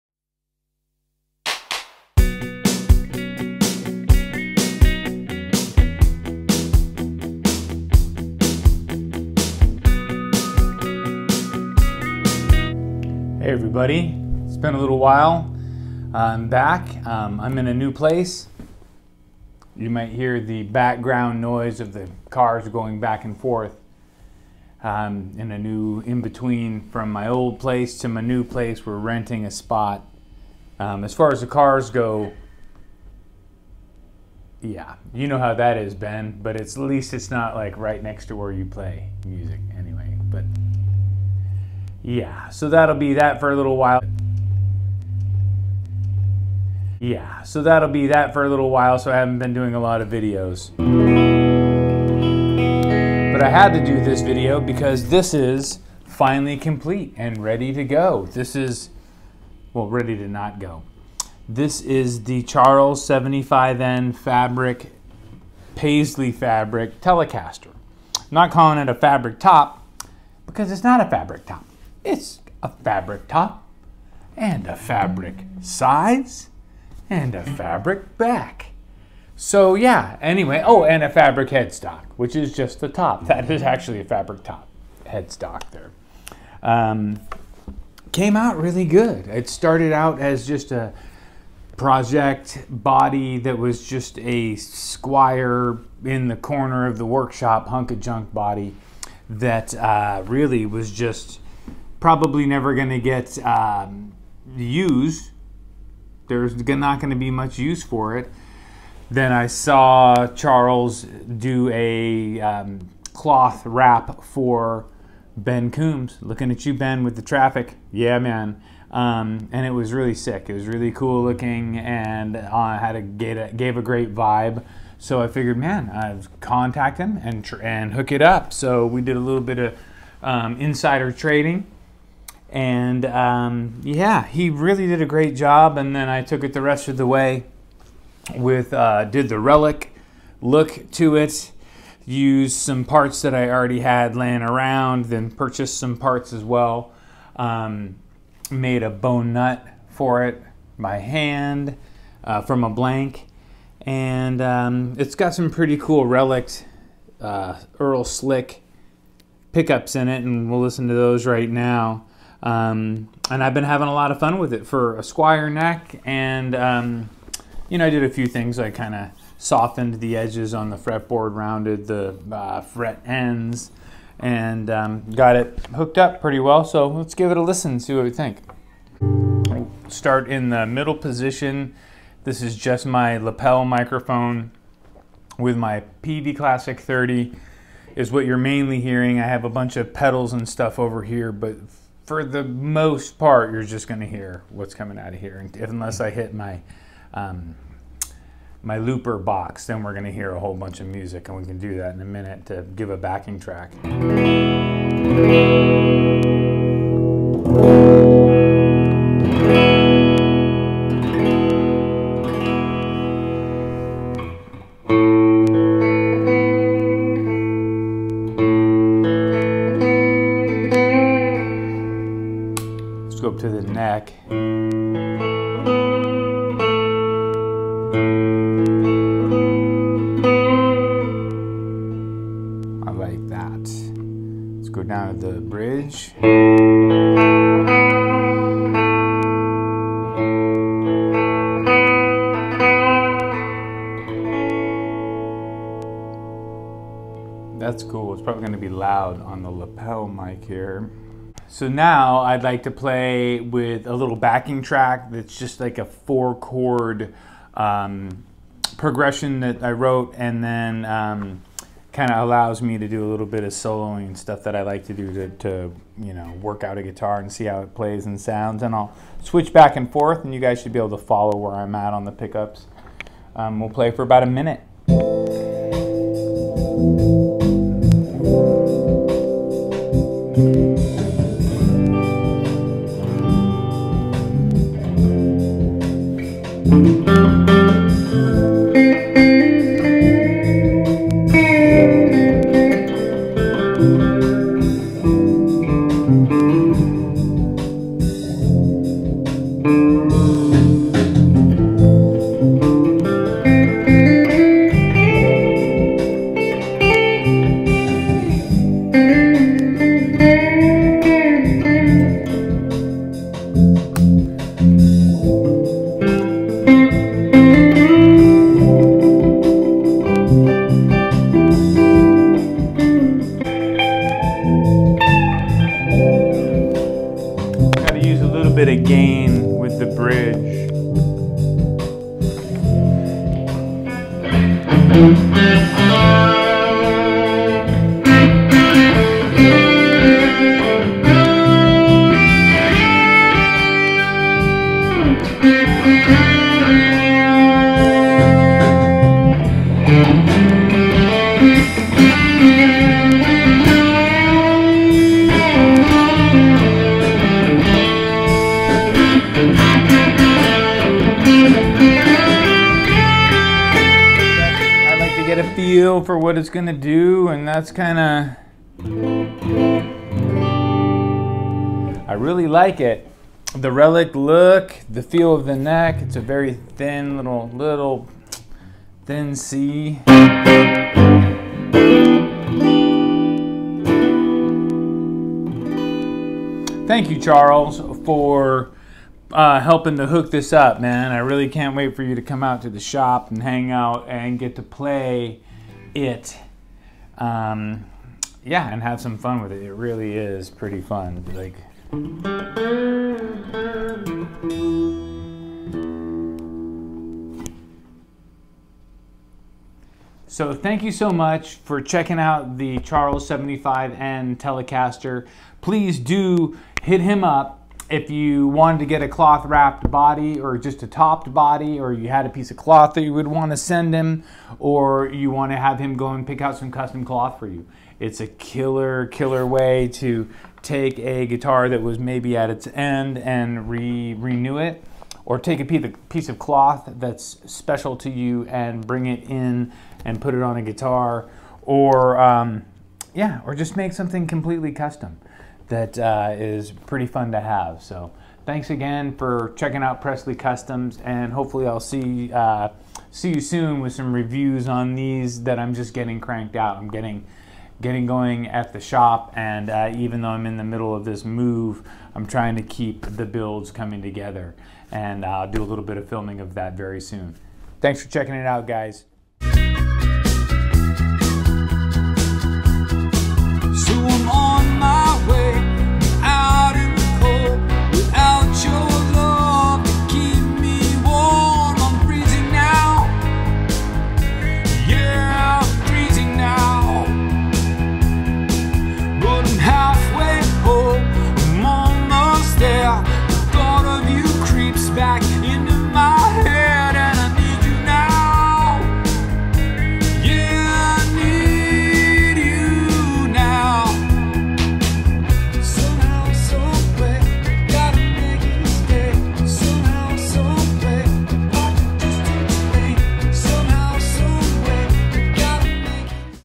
Charles 75N Fabric Tele is finally complete!!!
Playing, enjoying, and demonstrating the Charles 75N / Pressley Customs paisley Tele reveal